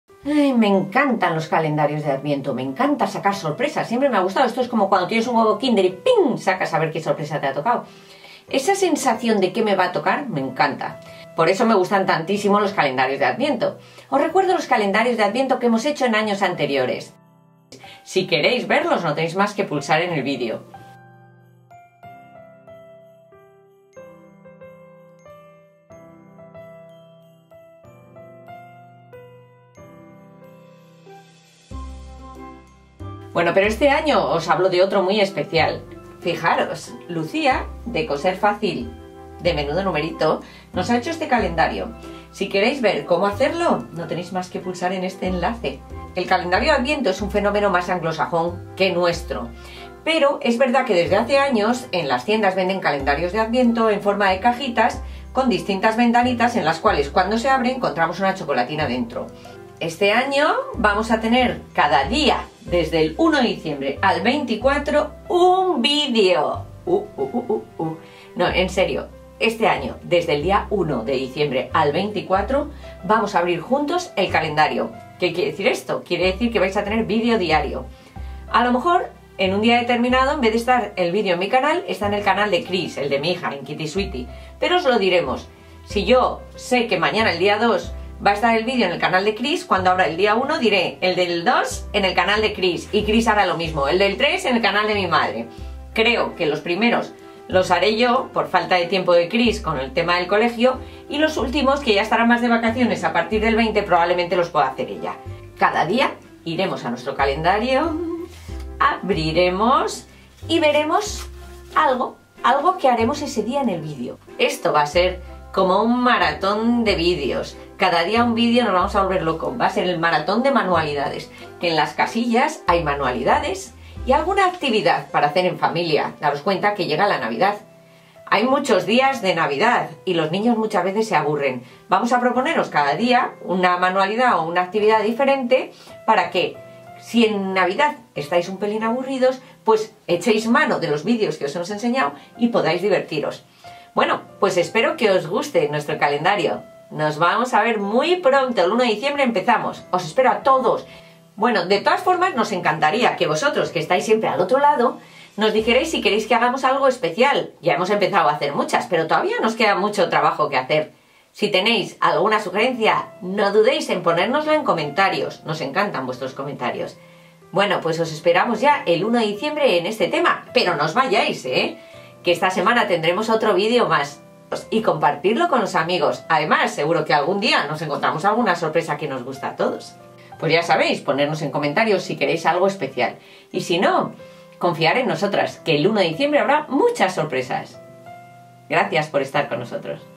Sorpresa: maratón de vídeos en Adviento
Vamos a tener un calendario de adviento para todos nosotros. Cada día, del 1 al 24 de diciembre miraremos lo que hay en cada casilla juntos y realizaremos alguna manualidad o actividad para que os pueda servir para las fiestas navideñas. Os parece bien?

además nos encantaría que le dieras LIKE y compartas¡